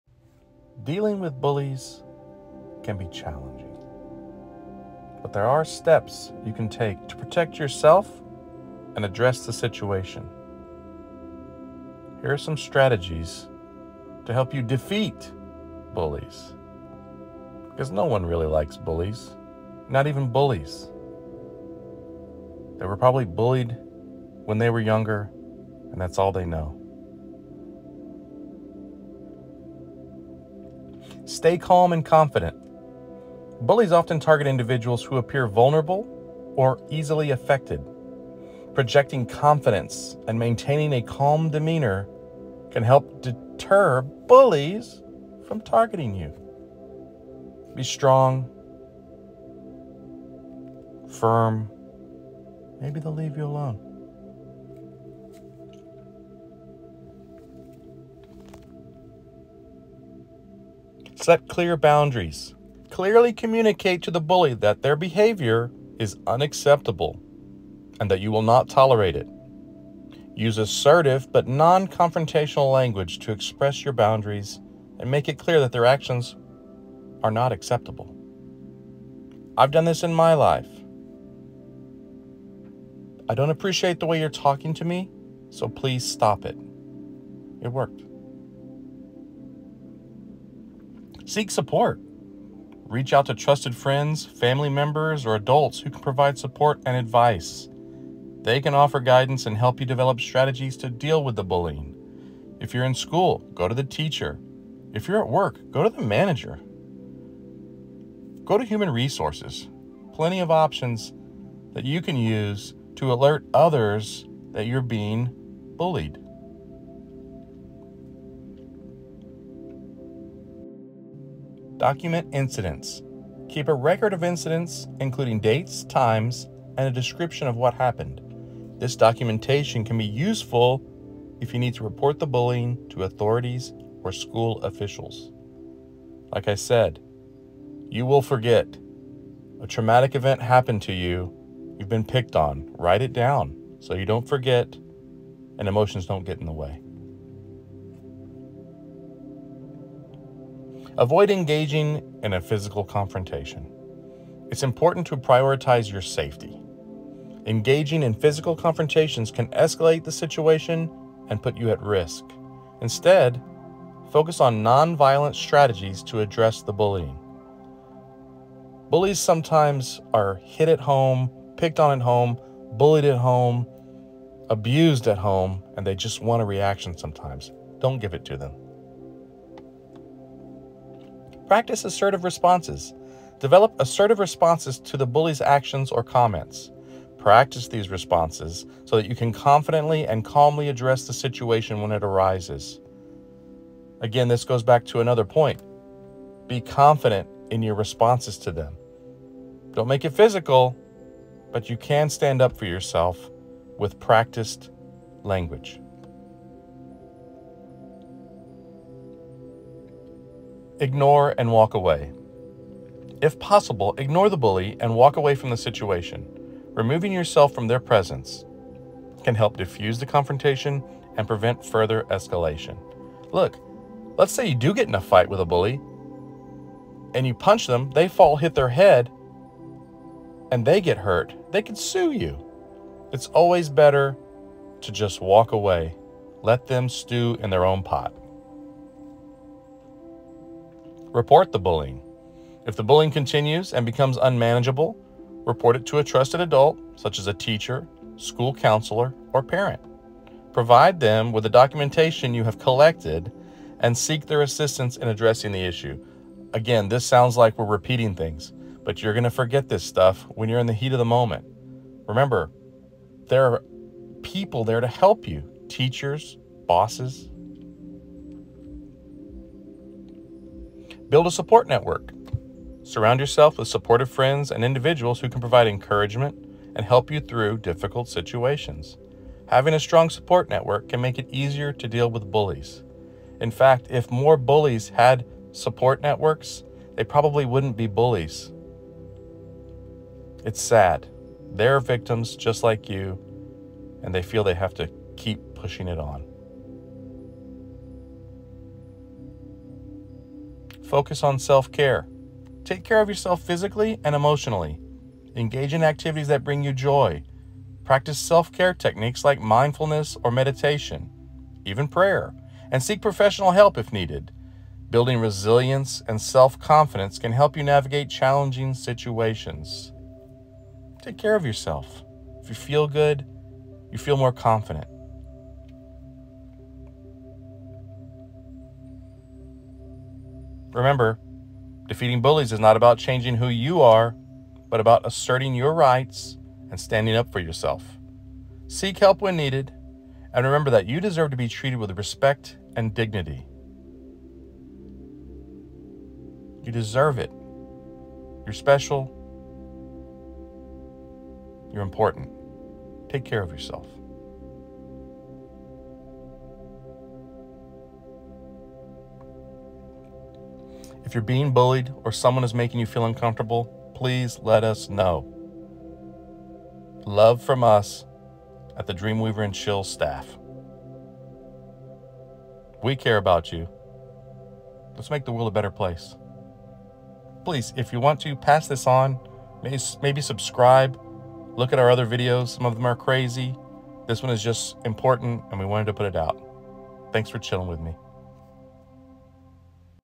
Dealing with bullies
A practical guide to dealing with bullies. I’m trying to help people deal with life when I can.